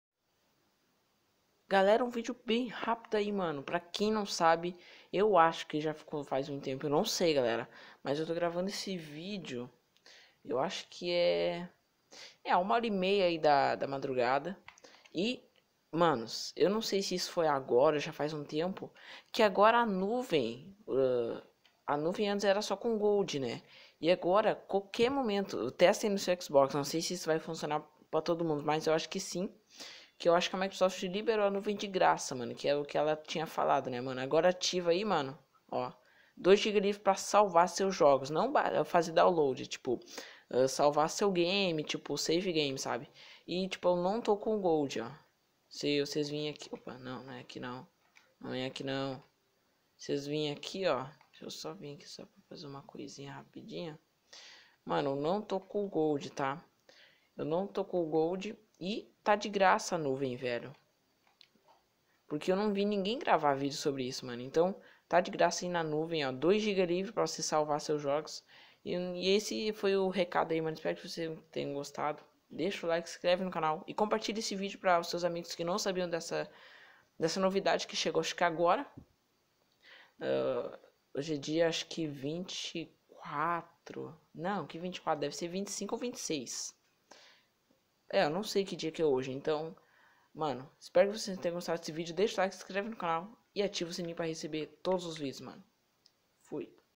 URGENTE!!!!NUVEM DO XBOX 360 ESTA DISPONIVEL PARA QUEM NÃO TEM GOLD!!!!!!!!CORREEE!!!!!!!!😱😱😱
Galera a nuvem do xbox 360 acabou de ficar disponível para quem não tem Xbox live Gold. então corre pra ativar ela.